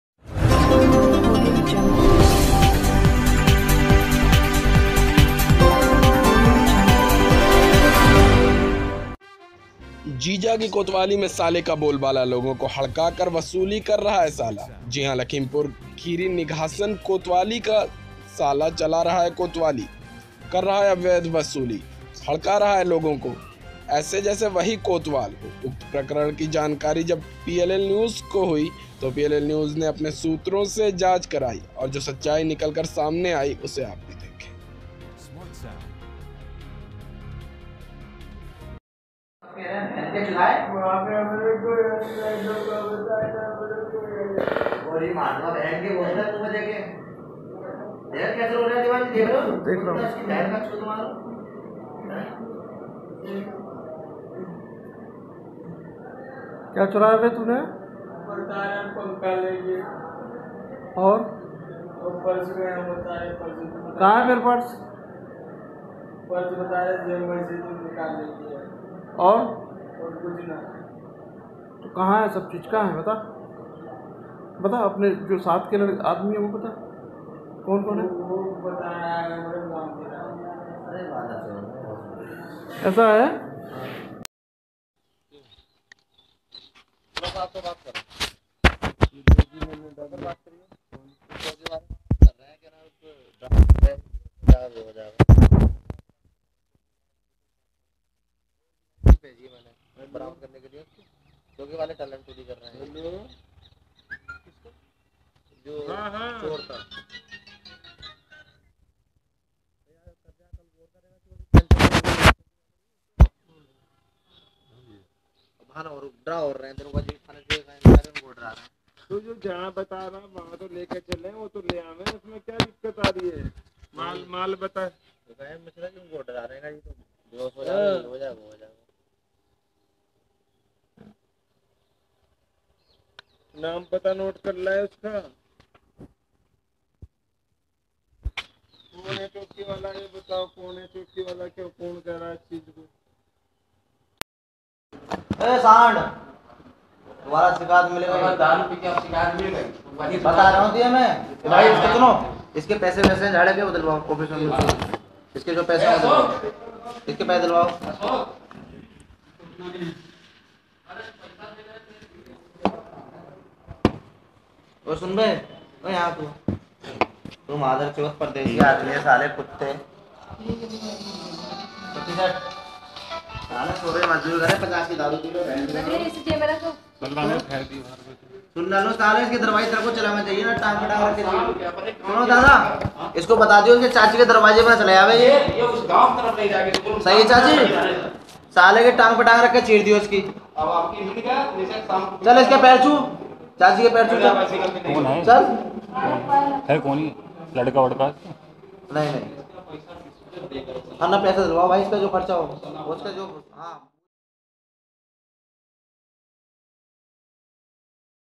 जीजा की कोतवाली में साले का बोल बाला || PLL News
जीजा की कोतवाली में साले का बोल बाला

लोगों को हड़का कर वसूली कर रहा है साला जी हां लखीम पुर निघासन कोतवाल का सला चला रहा है कोतवाली कर रहा है अवैध वसूली हड़का रहा है लोगों को ऐसे जैसे वहीं कोतवाल हो उक्त प्रकरण की जानकारी जब पी एल एल न्यूज़ को हुई तो पी एल एल न्यूज़ ने अपने सूत्रों से जांच कराई और जो सच्चाई निकल कर सामने आई उसे आप भी देखें